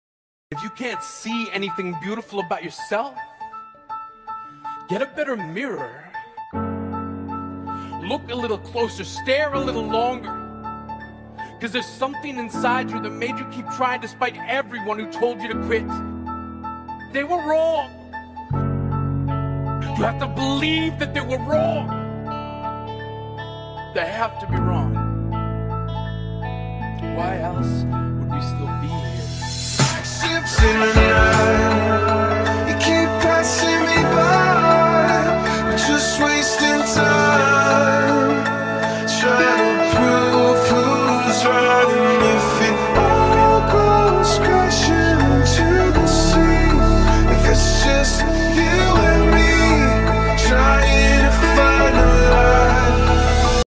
Ships in the night
we went to hire the school down the road, so be prepared for a better video from there! Unfortunately Talk was a little bit lame after jumping a bit so i stopped and just filmed lily! enjoy x